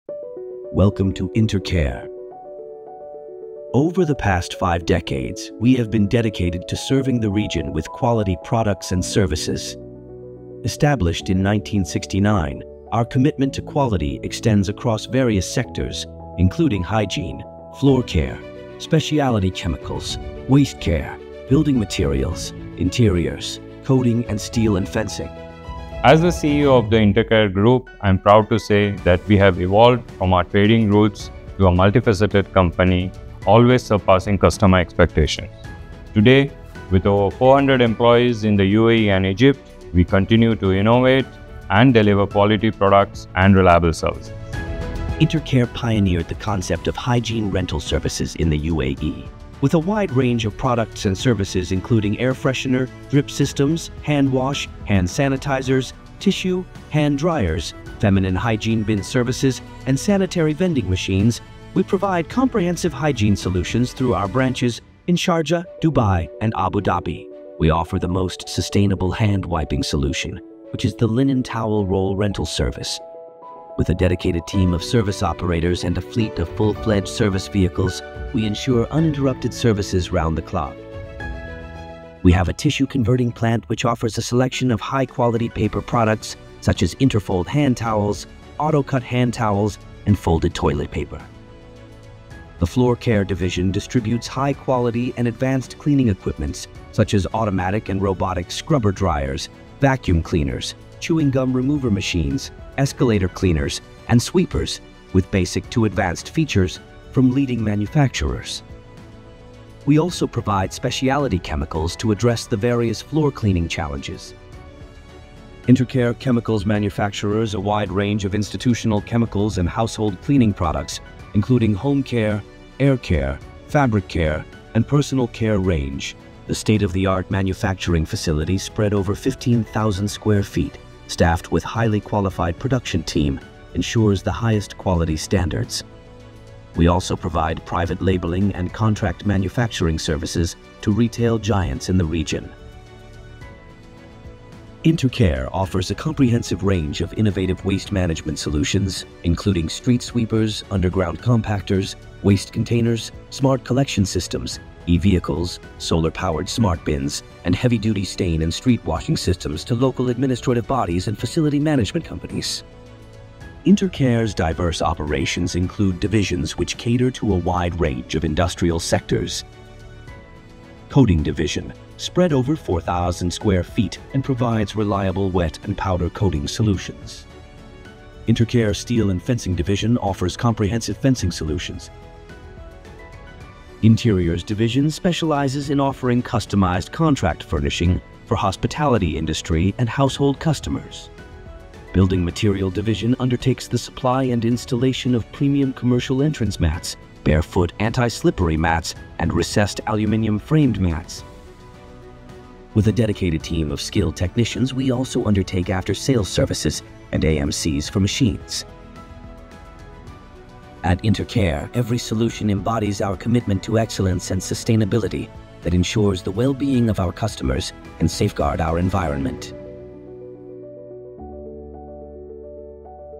Intercare Corporate 2024 | 50 Years of Care
At Intercare Limited, we don't just embrace change—we drive it. Our enduring values are not only the foundation of our success but also the compass guiding us towards a future of endless possibilities.
Watch as we showcase the diverse tapestry of our operations, united by a shared passion for excellence and a relentless pursuit of growth. Together, we have cultivated a cultureture where inclusivity thrives, empowering every individual to contribute their unique perspectives and talents.